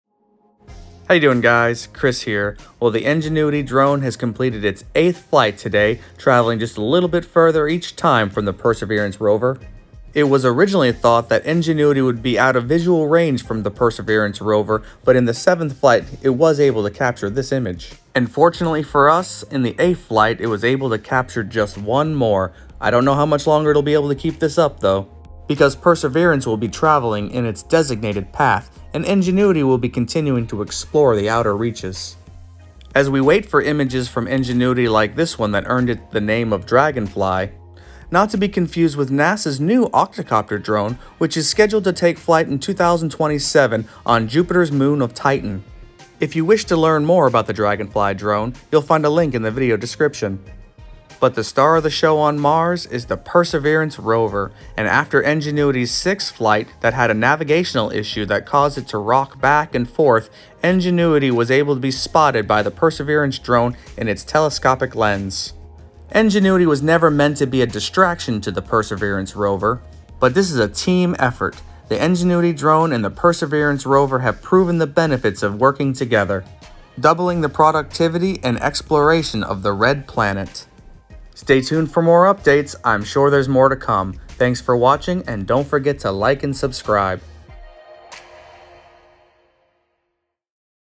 Ingenuity Spotted Flying During 8th Flight
The Ingenuity Helicopter Drone was seen in flight during its eighth mission on Mars.

Photo Credit: NASA